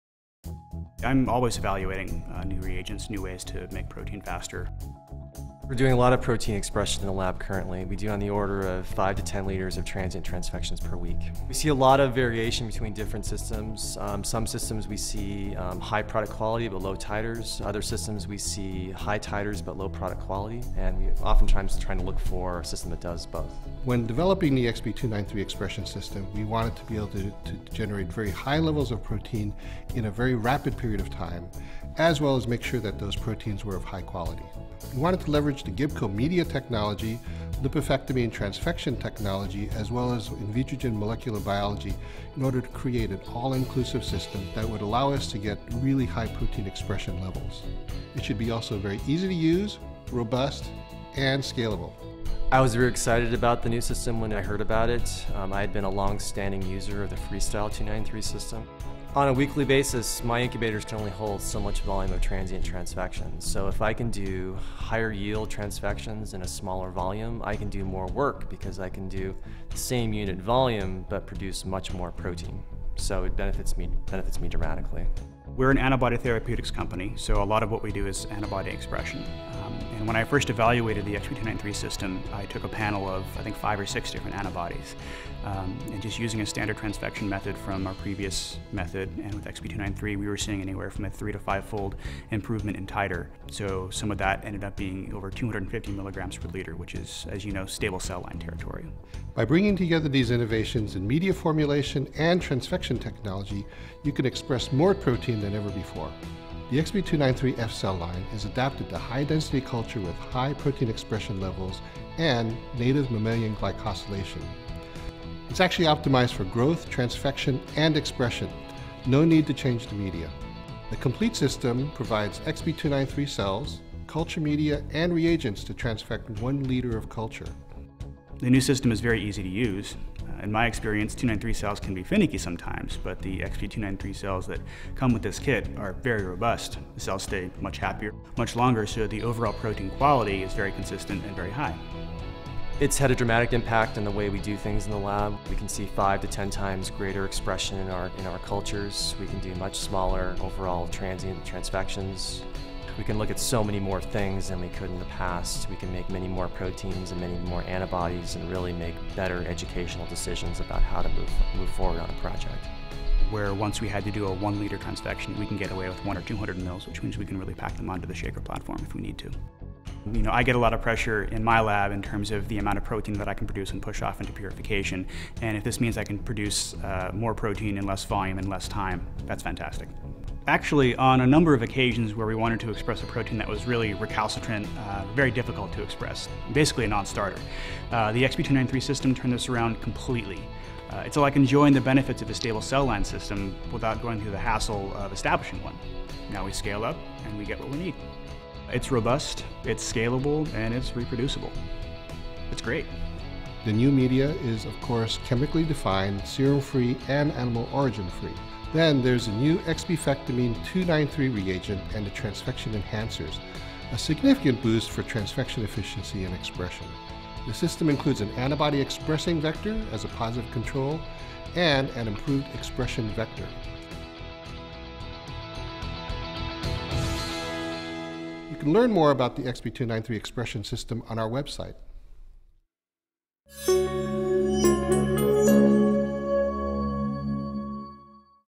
Transient Protein Expression - Expi293™ Expression System
Learn more about the next generation system for transient protein expression

The Expi293™ Expression System leverages Gibco® media technology, Lipofectamine™ transfection technology, and Invitrogen™ molecular biology, allowing you to generate the highest levels of high quality protein from mammalian cells in less than a week.

[Audio transcript:]

- I'm always evaluating new reagents, new ways to make protein faster.

- We are doing a lot of protein expression in the lab currently, we do on the order of 5 to  10 liters of transient transfection per week.  We see a lot of variation between different systems. Some systems we see high product quality, but low titers. Other systems we see high titers, but low product quality. And we often times try to look for a system that does both.

- When developing the Expi293™ Expression System, we wanted to be able to generate very high levels of protein in a very rapid period of time, as well as make sure that those proteins were of high quality. We wanted to leverage the Gibco® media technology, Lipofectamine™ transfection technology, as well as Invitrogen™ molecular biology in order to create an all-inclusive system that would allow us to get really high protein expression levels. It should be also very easy to use, robust, and scalable.

- I was really excited about the new system when I heard about it. I had been a long standing user of the FreeStyle™ 293 system. On a weekly basis, my incubators generally hold so much volume of transient transfection, so if I can do higher yield transfections in a smaller volume, I can do more work because I can do the same unit volume but produce much more protein. So it benefits me, benefits me dramatically.

- We're an antibody therapeutics company, so a lot of what we do is antibody expression. And when I first evaluated the Expi293™ system, I took a panel of I think 5 or 6 different antibodies, just using a standard transfection method from our previous method, and with the Expi293™ we were seeing anywhere from a 3 to 5 fold improvement in titer.  So some of that ended up being over 250 mg per liter, which is - as you know - stable cell line territory.

- By bringing together these innovations in media formulation and transfection technology, you can express more protein than ever before. The Expi293F™ Cell line is adapted to high-density culture, with high protein expression levels, and native mammalian glycosylation.  It is actually optimized for growth, transfection AND expression - no need to change media!  The complete system provides Expi293™ cells, culture media and reagents to transfect 1 liter of culture.

- The new system is very easy to use. In my experience, 293-cells can be finicky sometimes, but the Expi293™ cells that come with this kit are very robust. The cells stay much happier much longer so the overall protein quality is very consistent and very high.

- It's had a dramatic impact on the way we do things in the lab. We can see 5 to 10 times greater expression in our cultures. We can do much smaller, overall, transient transfections.  We can look at so many more things than we could in the past. We can make many more proteins, and many more antibodies, and really make better educational decisions about how to move forward on a project.

- Where once we had to do a 1 liter transfection we can get away with 1-200 mL which means we can really pack them on to the shaker platform if we need to. You know, I get a lot of pressure in my lab in terms of the amount of protein that I can produce and push off into purification, and if this means I can produce more protein in less volume and less time, that's fantastic! Actually, on a number of occasions where we wanted to express a protein that was very recalcitrant, very difficult to express -- basically a non-starter --the Expi293™ system turned this around completely. It's like enjoying the benefits of a stable cell line system, without going through the hazzle of establishing one. Now we scale up and we can get what we need. It's robust, it's scalable, and it's reproducible . It's great.

- The new media is of course chemically defined, serum-free, and animal origin-free. Then there's the new ExpiFectamine™ 293 Reagent and the Transfection Enhancers - a significant boost for transfection efficiency and expression.  The system includes an antibody expressing vector as a positive control, and an improved expression vector.

You can learn more about the Expi293™ Expression System on our website.